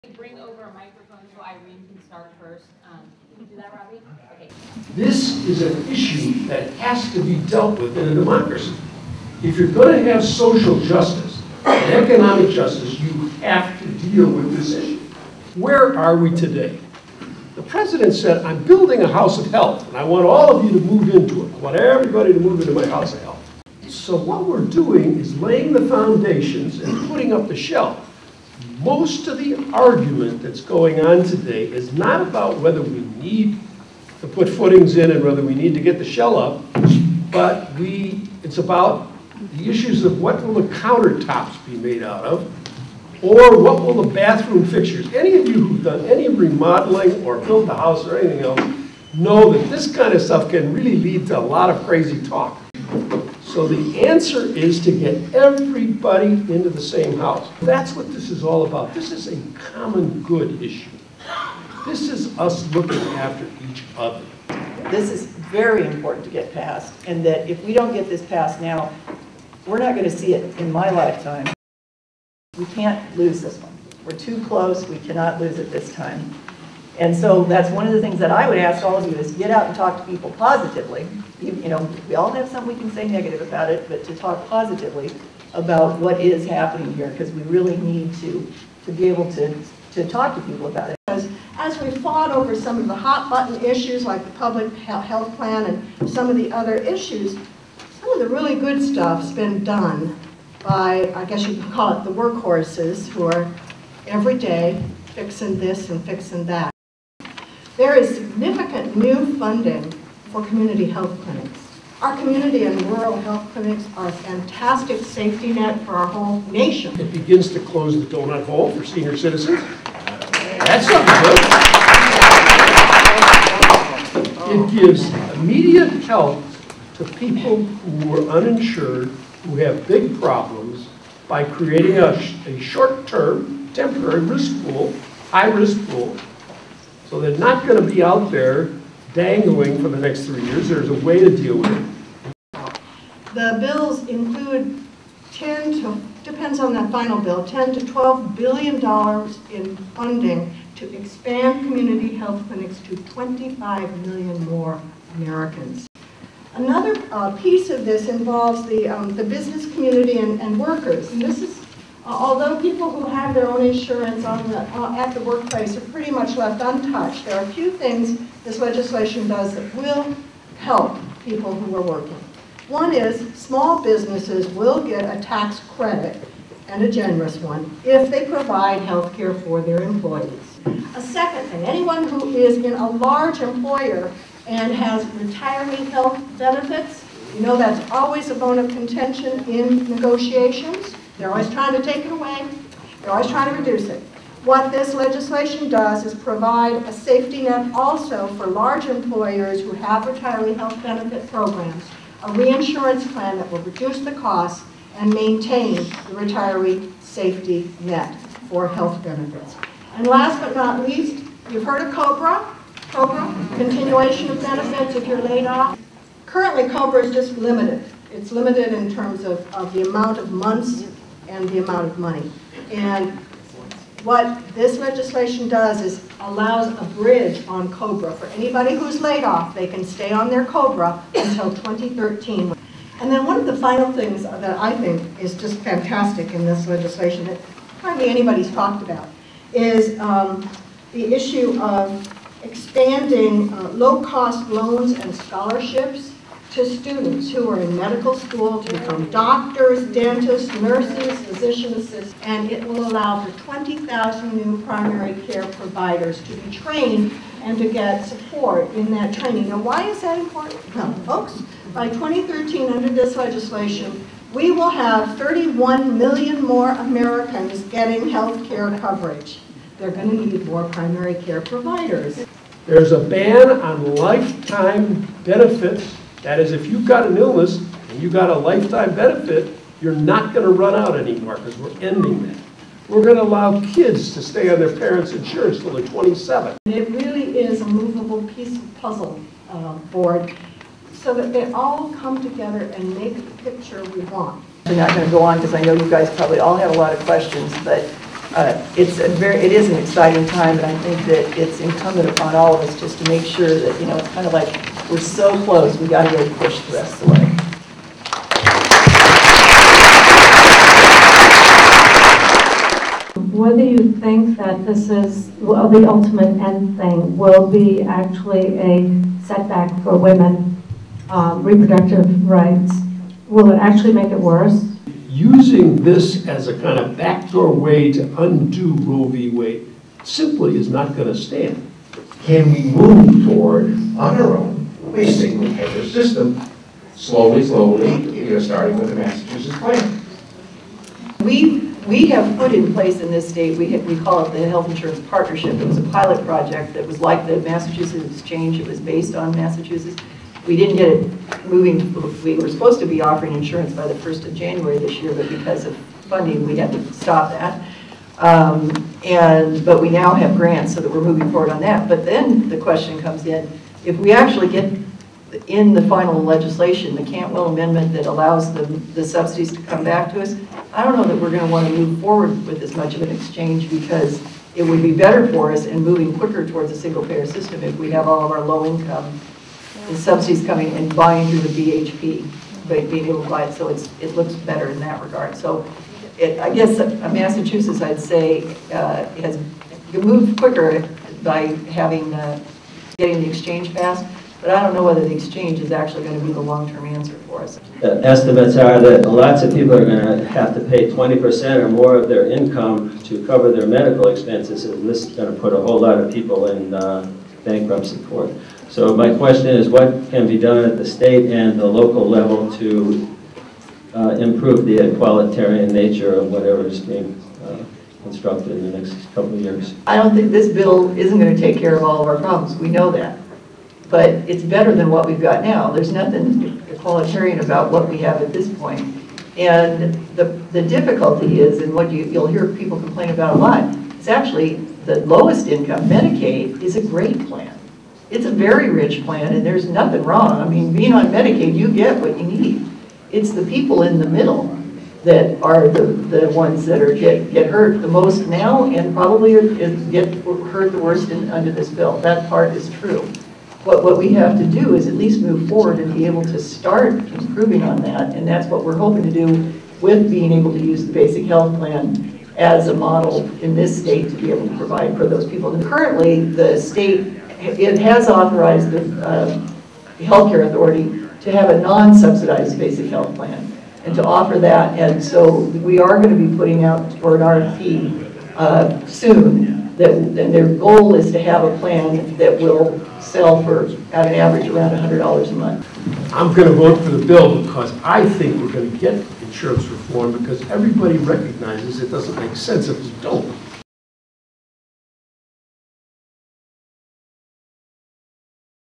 HWC_forum_Jan17.avi
The Healthy Washington Coalition held a forum on Jan. 17, 2010 to answer the question: "Where are we now in reforming the health insurance system?" More than 400 people packed the hall to listen to Representative Jim McDermott, State Senator Karen Kaiser and Representative Eileen Cody. They discussed the good provisions in the health insurance reform bills and answered questions.